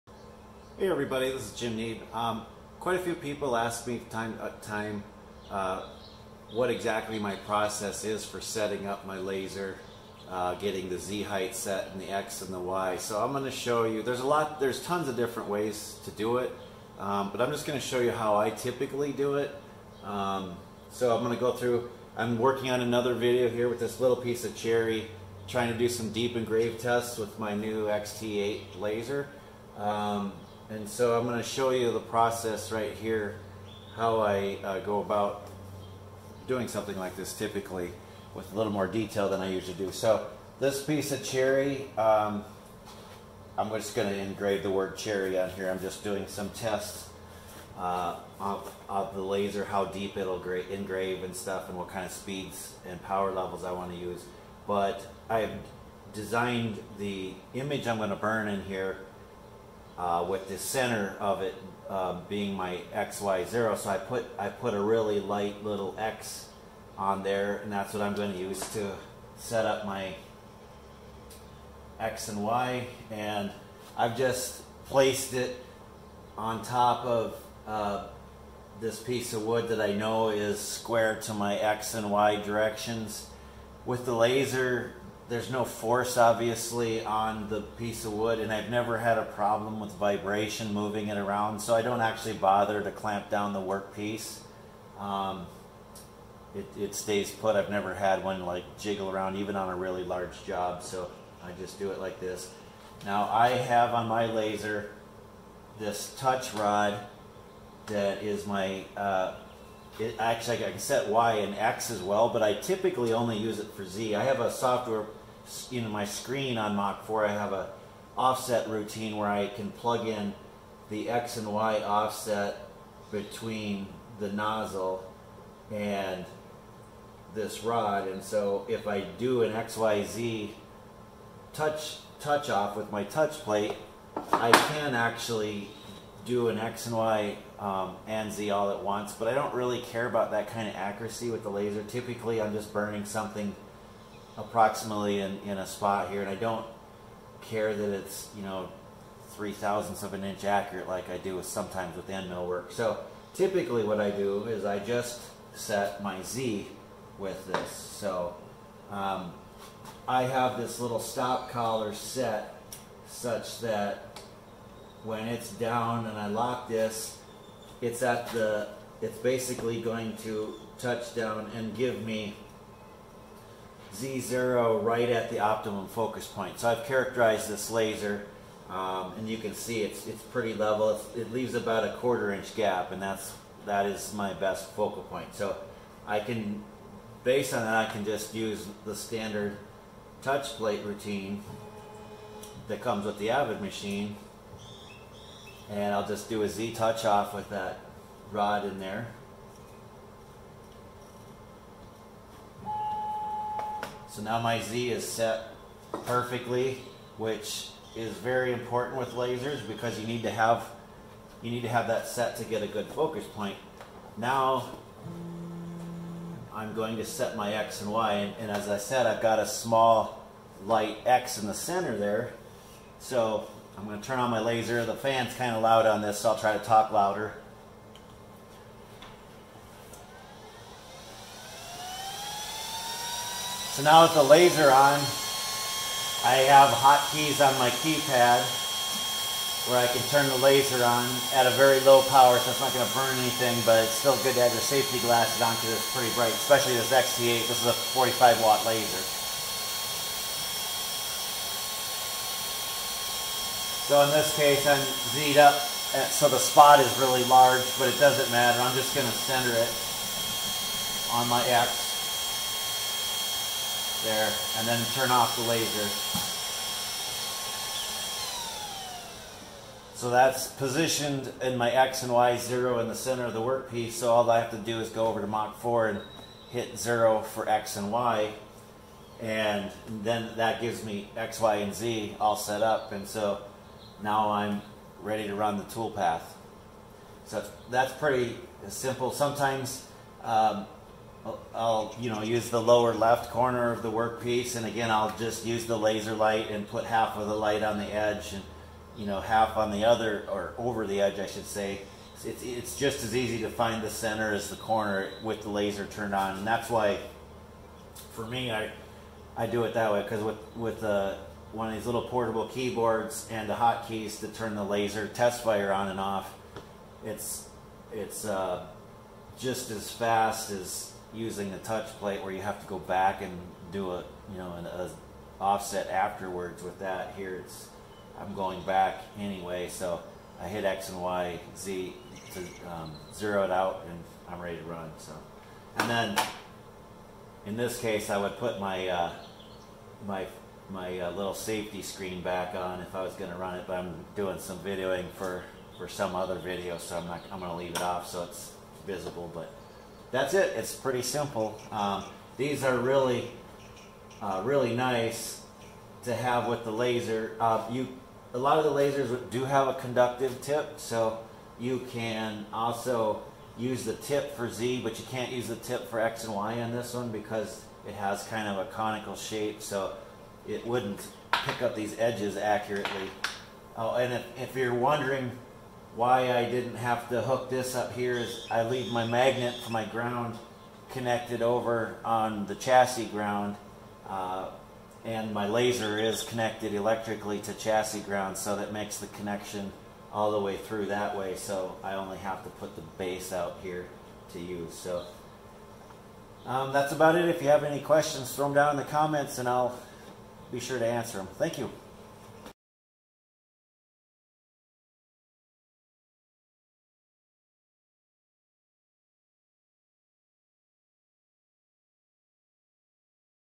Laser Job Setup on my CNC
This is a quick overview of the setup process I use for each laser job with my Opt lasers on my AVID Pro CNC

More information on the Opt XT8 laser can be found